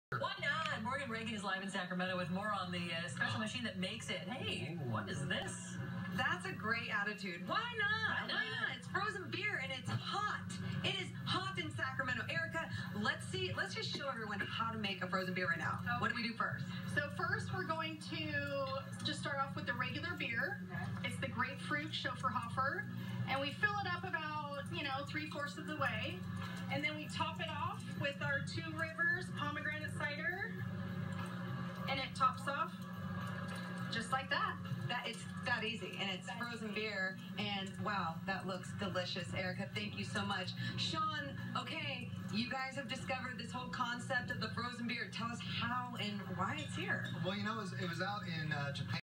Frozen Beer Hits Sacramento
Frozen beer hits Sacramento at Der Biergarten in Midtown Sacramento. Gret German food, 32 Beers on tap and now Frozen Beer!